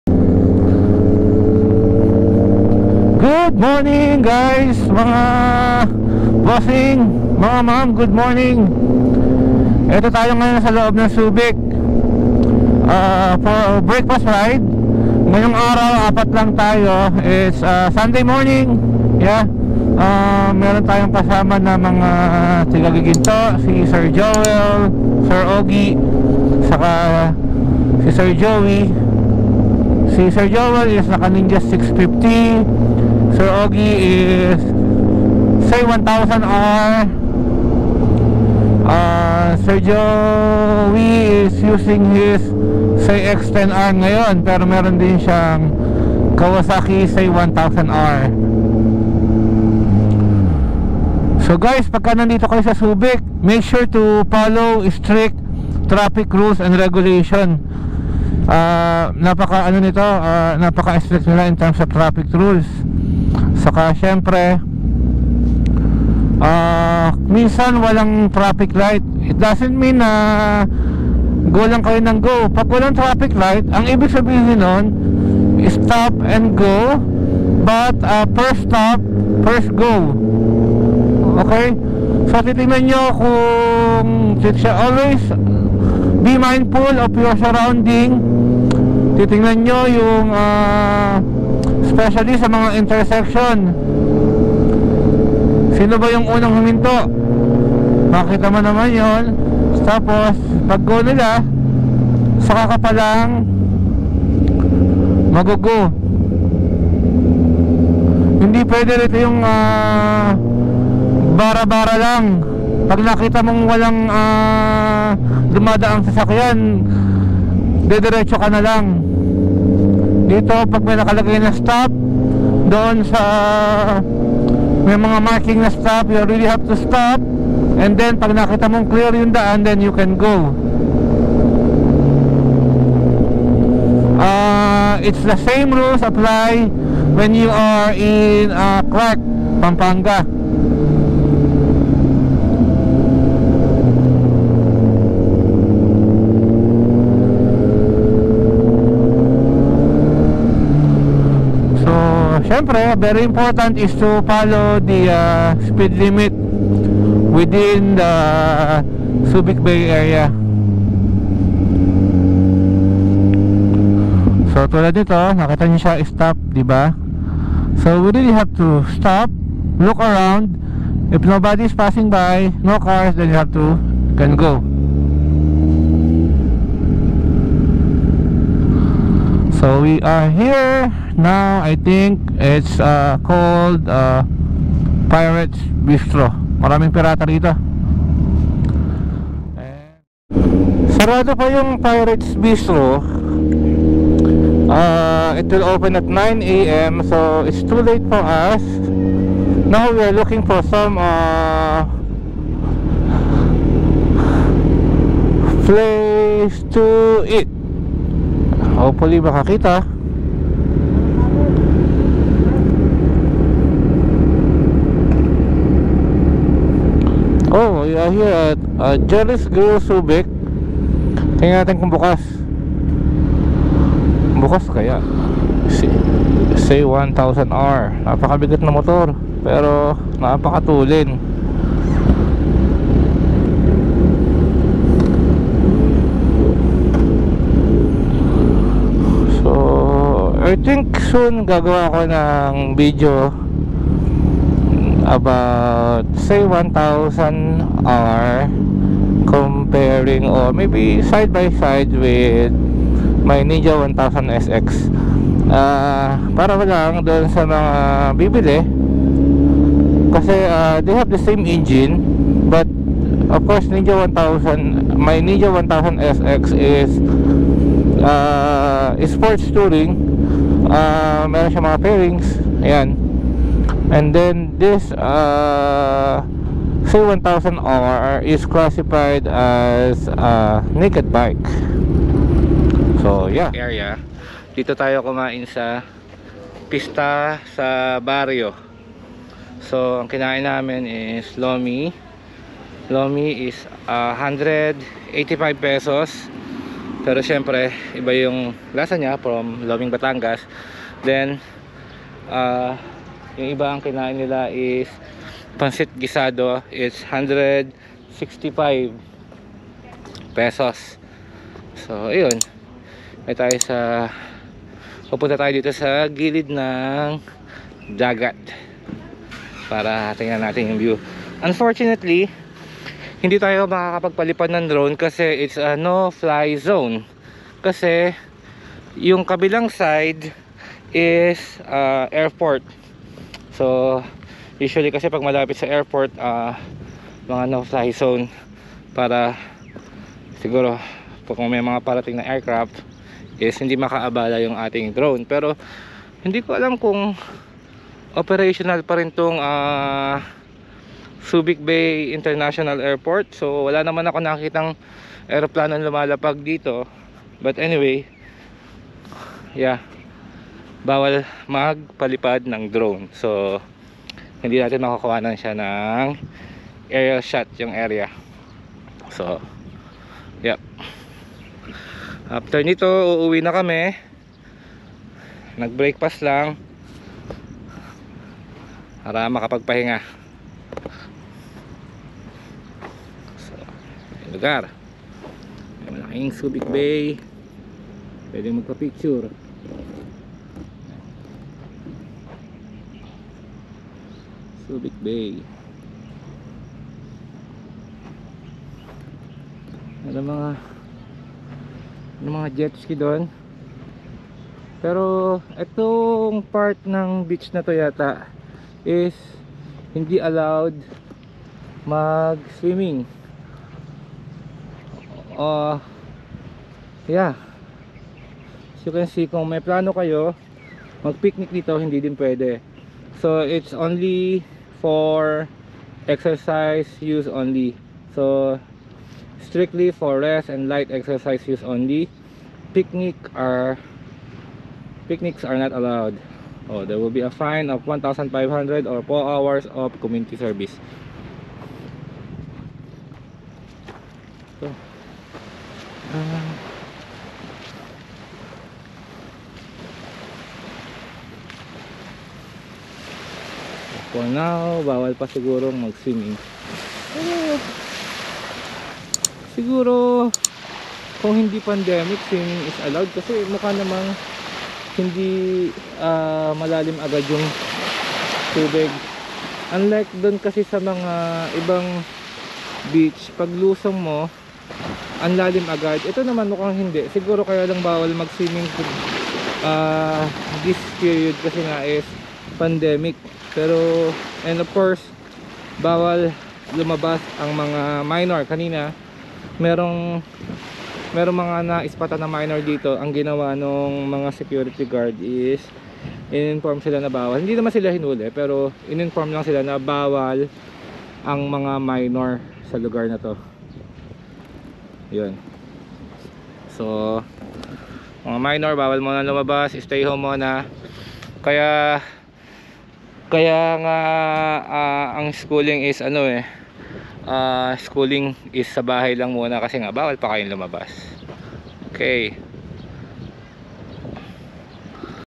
Tips on Riding Inside Subic Bay I Kawasaki Ninja 1000sx I Junnel Santos TV I Moto-trip
Tips on Riding Inside Subic Bay I Breakfast Ride @ Pista Sa Barrio I Junnel Santos TV I Moto-trip. I also shared some Guide on Basic Traffic Rules inside Subic and Clark.